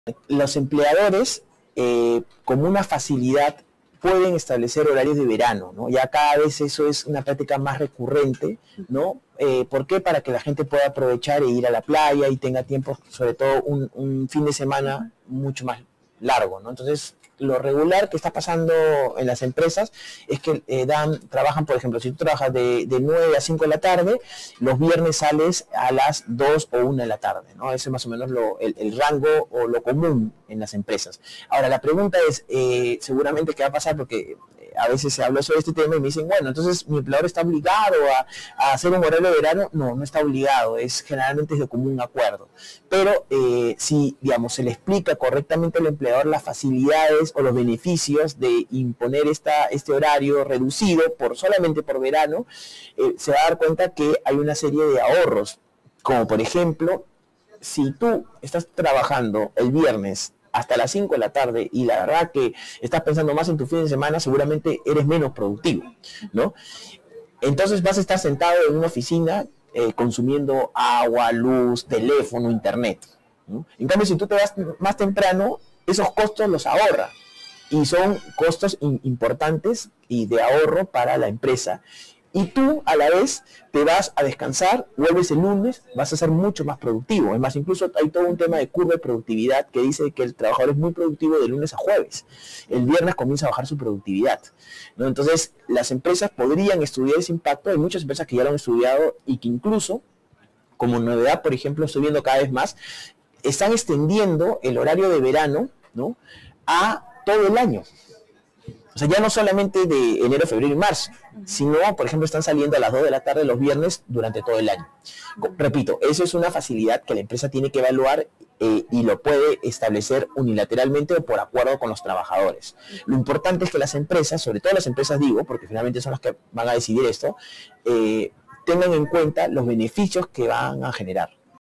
¿Cuáles son los beneficios del horario de verano?
¿Cuáles son los beneficios del horario de verano? Brian Ávalos, abogado laboralista del estudio Payet, Rey, Cauvi, Pérez, explicó más sobre ello.